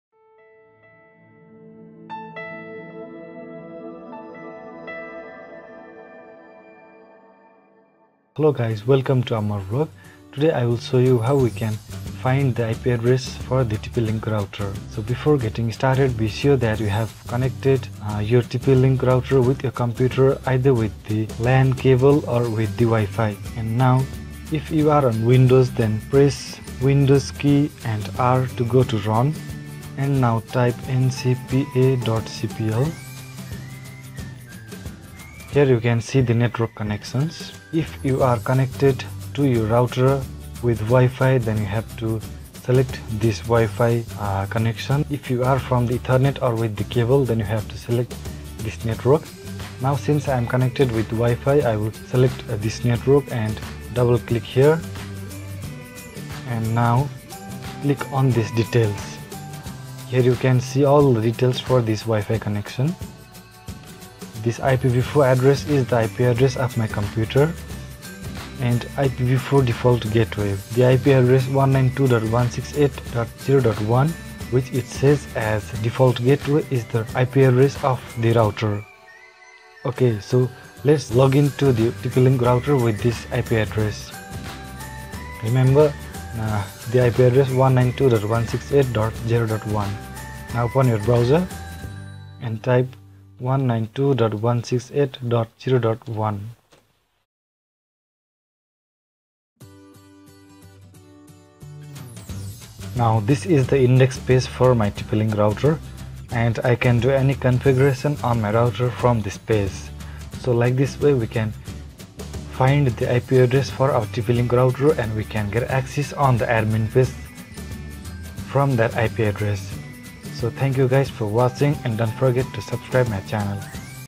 How to Find IP Address of TP-Link Router ✔
This video helps you to find the default IP Address of TP-Link router. Most of the TP-Link Router have printed their IP address on the bottom of their Router Device but if someone has changed the IP address then you can view the password from this method.
If you want to change the IP Address of your TP-Link Router then check this video